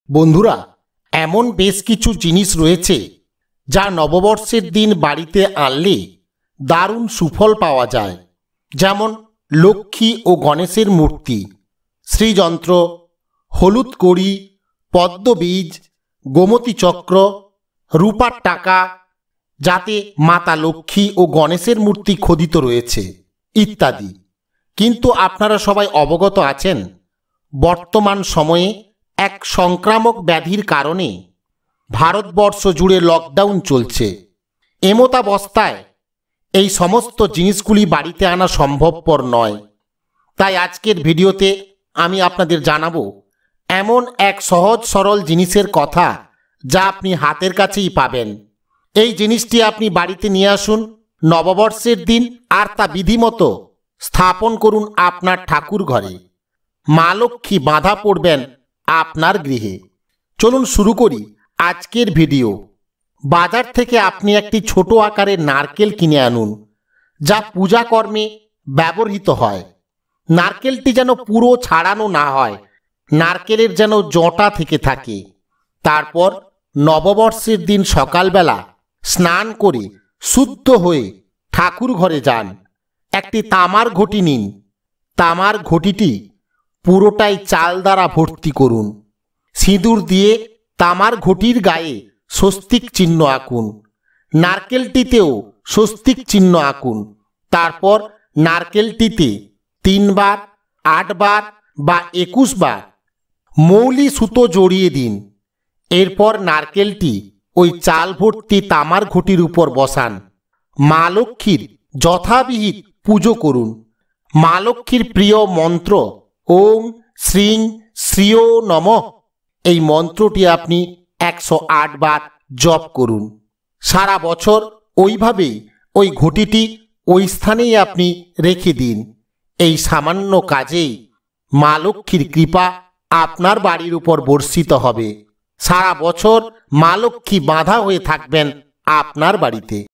নববর্ষের দিন বাড়িতে আনুন এই একটি জিনিস মা লক্ষ্মী বাঁধা থাকবেন আপনার ঘরে
এমন কিছু জিনিস রয়েছে যা নববর্ষের দিন বাড়িতে আনা খুবই লাভদায়ক। যেমন লক্ষ্মী গণেশের মূর্তি, হলুদ কড়ি, শ্রী যন্ত্র, পদ্মবীজ, গোমতি চক্র, লাফিং বুদ্ধ, দক্ষিণাবর্ত শঙ্খ ময়ূর পালক ইত্যাদি। কিন্তু বর্তমান বছরে এই জিনিস গুলি সংগ্রহ করা খুবই মুশকিল। তাই আজ আপনাদেরকে এই নতুন বছরে এমন এক  জিনিস বাড়িতে আনতে বলবো যা আনা খুবই সহজ ব্যাপার। তাই এই একটি জিনিস আপনি নববর্ষের দিন বাড়িতে আনুন মা লক্ষ্মী আপনার বাড়িতে বাঁধা হয়ে থাকবেন।